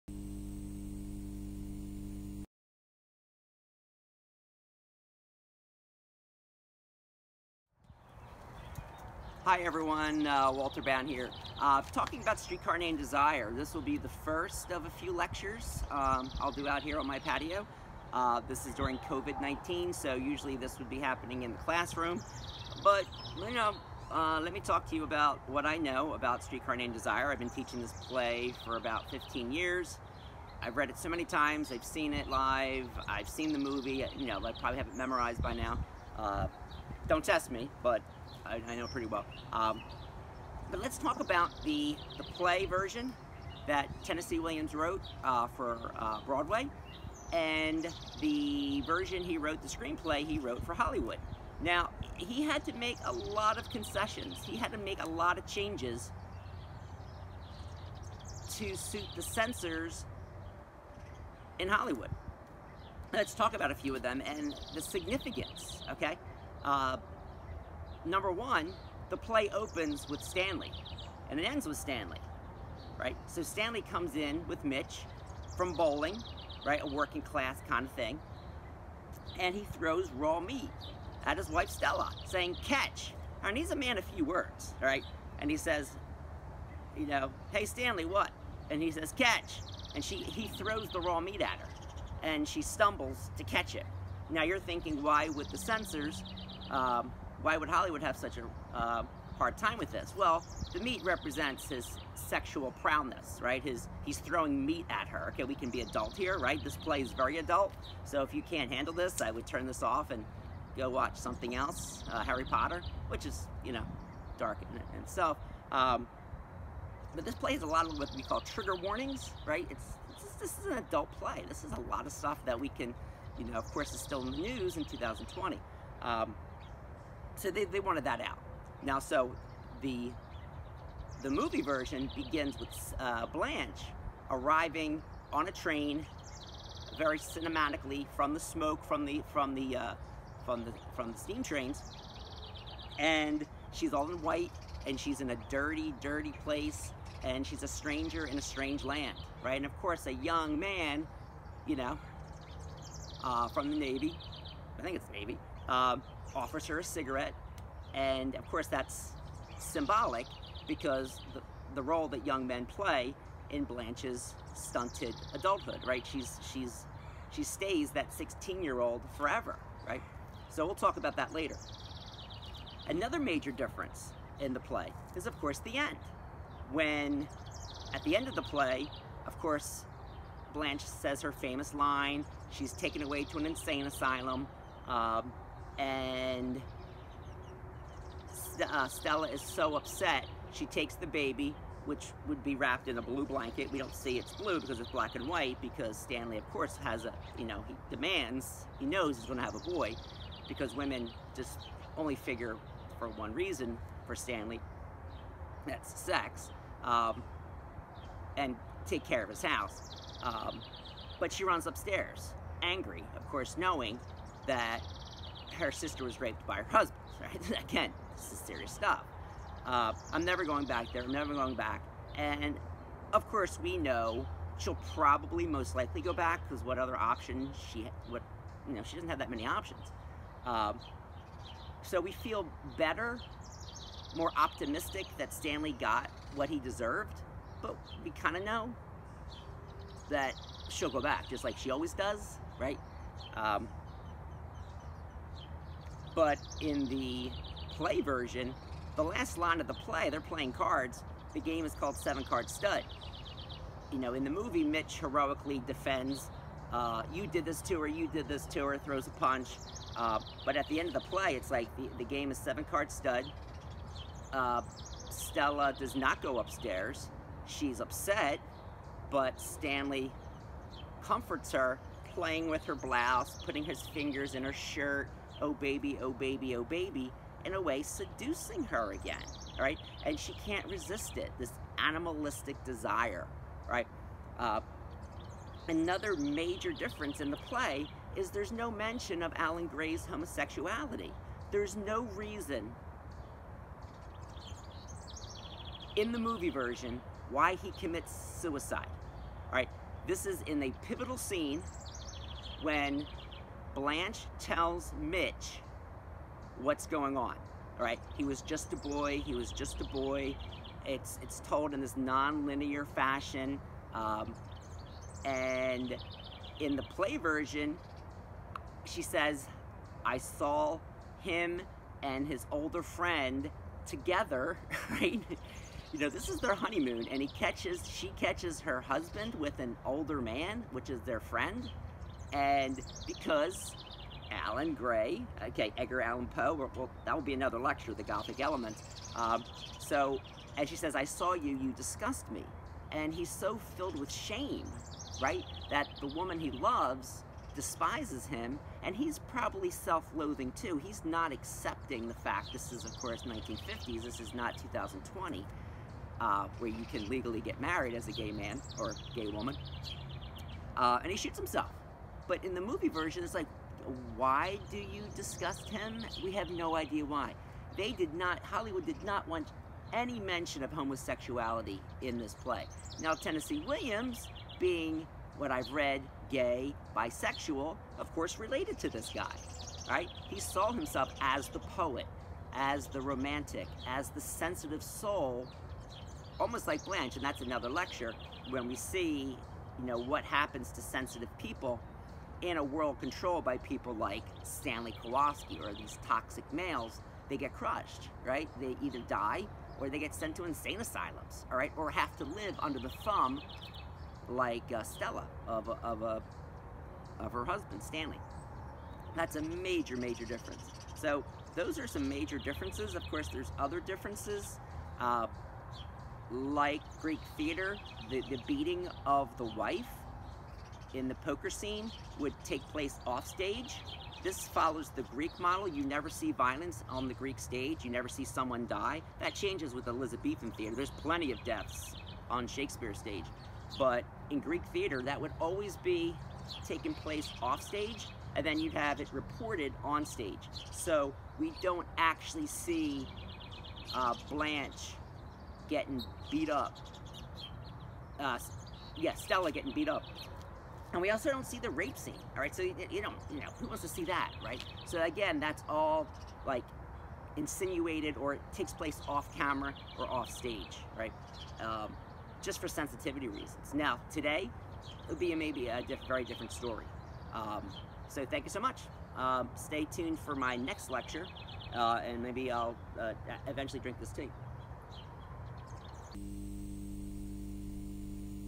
A Streetcar Named Desire: Broadway vs Hollywood
Tennessee Williams had to change essential parts of his Broadway play to suit the tastes of the censors and Hollywood execs. In this video lecture, I discuss those parts.